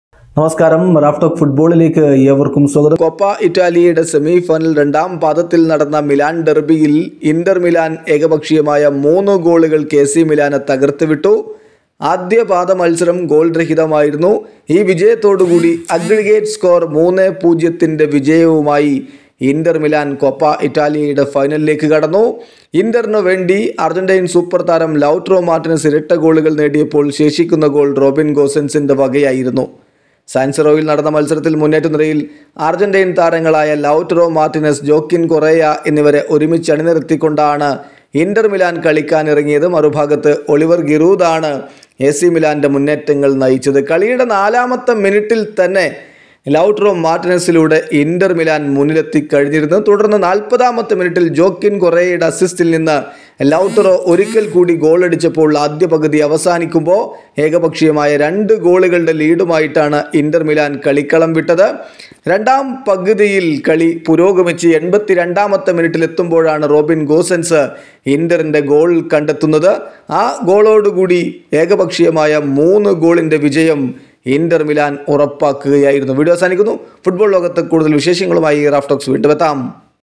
ലൗറ്ററോക്ക് ബ്രെയ്സ്, മിലാനെ തകർത്ത് ഇന്റർ ഫൈനലിൽ | Inter Milan vs AC Milan | Coppa Italia
Inter Milan vs AC Milan
Coppa Italia